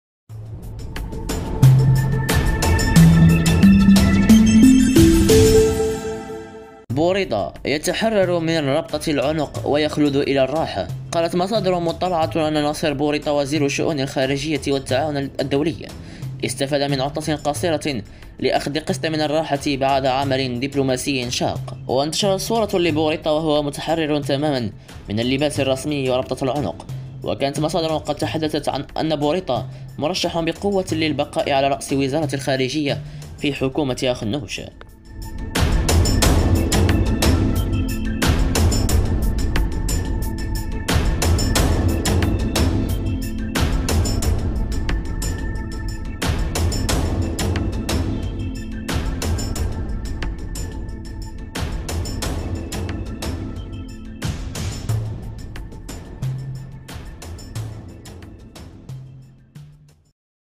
بوريطة يتحرر من ربطة العنق و يخلد إلى الراحة
بوريطة يتحرر من ربطة العنق و يخلد إلى
الراحة

قالت مصادر مطلعة أن ناصر بوريطة وزير الشؤون
الخارجية والتعاون الدولي ، استفاد من
عطلة قصيرة
الأخذ قسط الراحة بعد عمل دبلوماسي شاق.